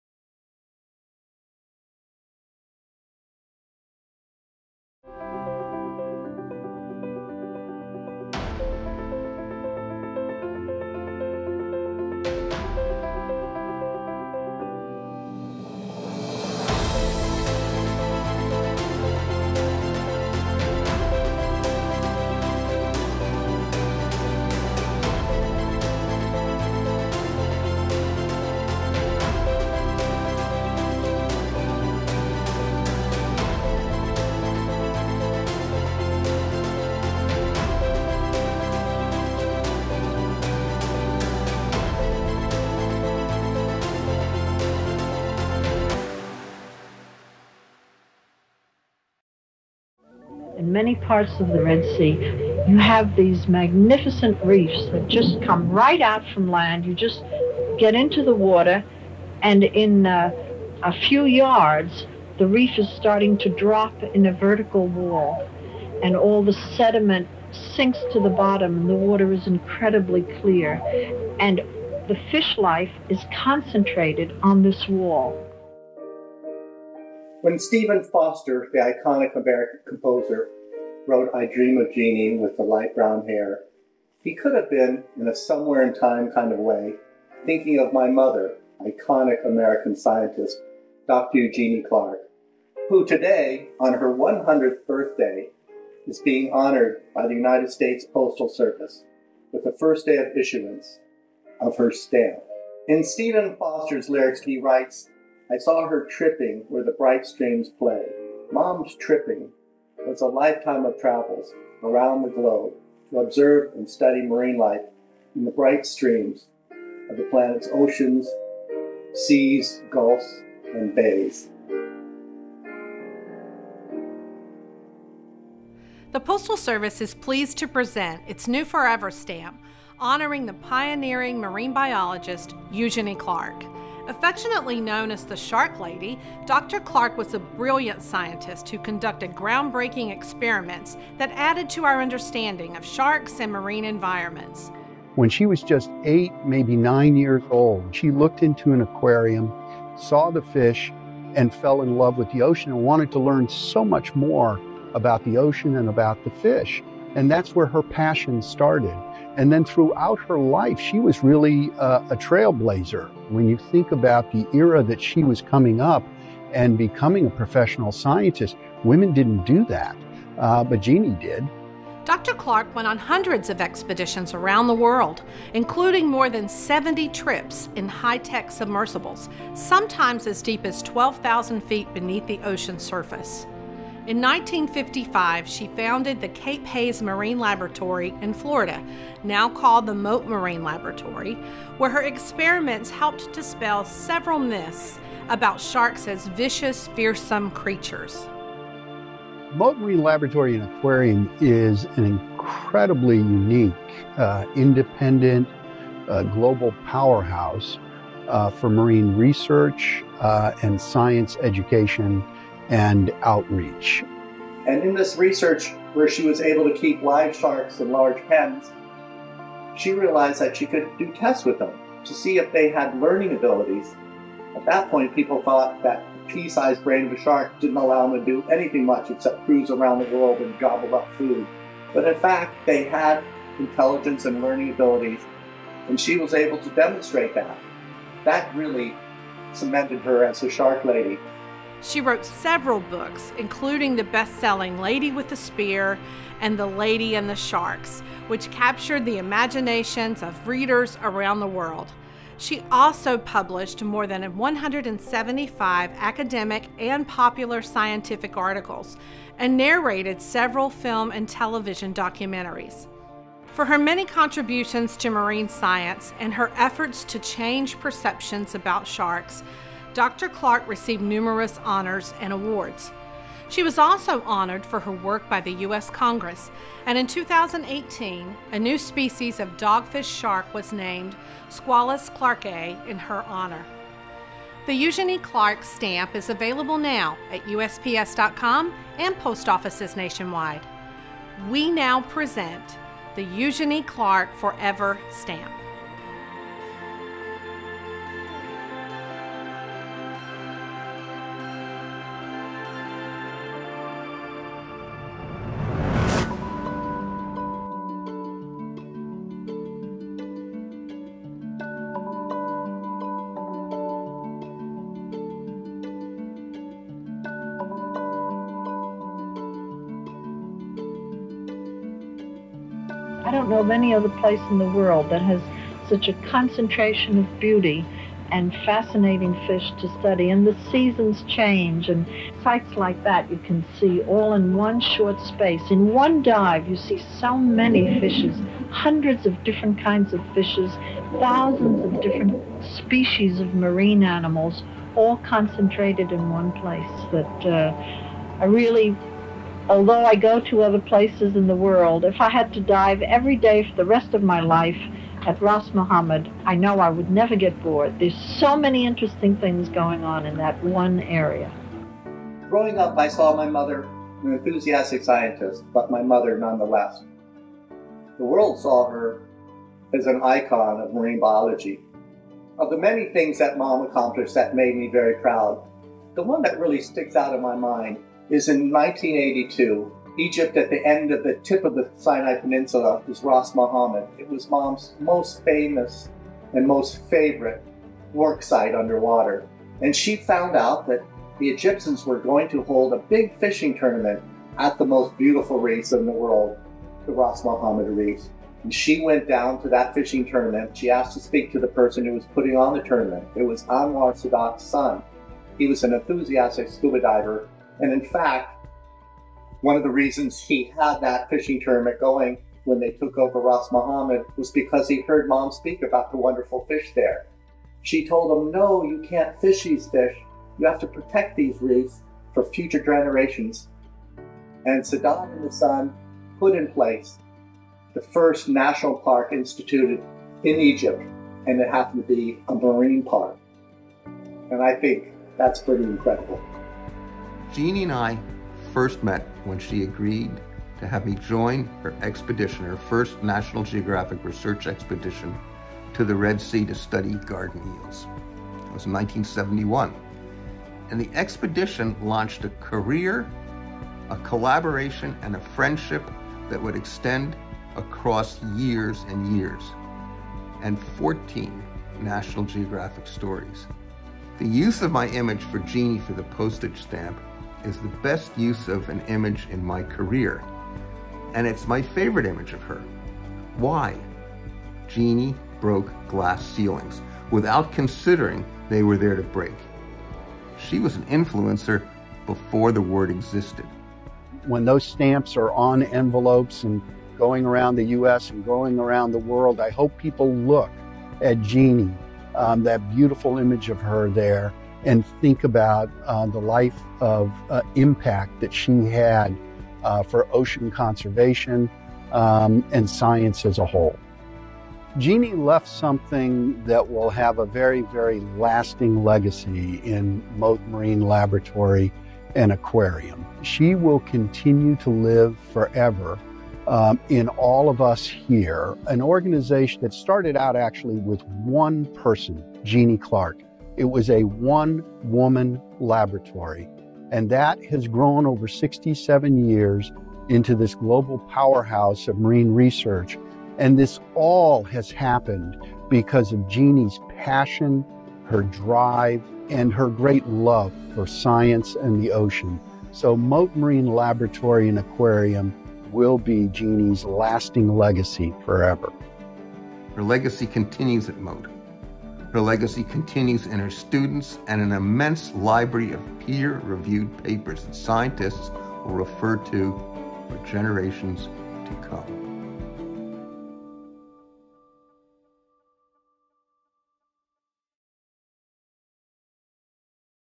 Eugenie Clark Commemorative Forever® Stamp
Eugenie Clark, a pioneering marine biologist who spent her career working tirelessly to change public perception about sharks — as well as to preserve marine environments around the world — was immortalized today on a Forever stamp.

The live dedication took place at the Mote Marine Laboratory & Aquarium, which Clark helped found as the Cape Haze Marine Laboratory in 1955.

“One of the goals of the Postal Service’s stamp program is to celebrate the people who represent the best of our nation and Eugenie Clark — I should say Dr. Clark or the ‘Shark Lady’ — certainly deserves this recognition,” said Angela H. Curtis, the Postal Service’s vice president of delivery operations and dedicating official. “She was a brilliant scientist whose groundbreaking work added to our understanding of sharks and marine environments.”

A prolific scientist animated by an unyielding sense of curiosity, Clark (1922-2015) carried out groundbreaking experiments and more than 200 expeditions across the globe. She demonstrated that lemon sharks could be trained to do complex tasks, disproved the notion that some shark species must keep swimming in order to survive, and debunked myths about sharks as vicious, fearsome creatures.

Clark was a pioneer in the era when scuba emerged as a research tool and later took more than 70 trips in high-tech submersibles, sometimes as deep as 12,000 feet beneath the ocean surface — something that has still been done by only a small number of other marine biologists.

“Our family is thrilled to see Dr. Clark, or as we call her, Grandma Genie, recognized and honored on a stamp. Her work as an ichthyologist was groundbreaking for proving that sharks are intelligent, and she was a pioneer for female scientists, researchers and scuba divers. We are so proud of her legacy as an Asian-American woman, teacher, scientist — and most importantly — grandmother,” said Aya Konstantinou, Clark’s daughter.

For her contributions to marine science, she received the Franklin L. Burr Award from the National Geographic Society, the Explorers Club Medal, and the Medal of Excellence from the American Society of Oceanographers, among numerous other recognitions. In 2015, she was honored posthumously by the U.S. Congress. In 2018, a newly discovered species of dogfish shark found in the Gulf of Mexico was named Squalus clarkae in her honor.

The stamp art features a digital collage created by multidisciplinary artist Amanda Phingbodhipakkiya. Her design includes a photograph of Clark taken by David Doubilet, as well as a photograph of a lemon shark taken by Reinhard Dirscherl. Wavy blue elements in the stamp’s background evoke an undersea scene.

The Eugenie Clark Forever stamp is being sold in panes of 20 stamps. News of the Eugenie Clark stamps is being shared with the hashtag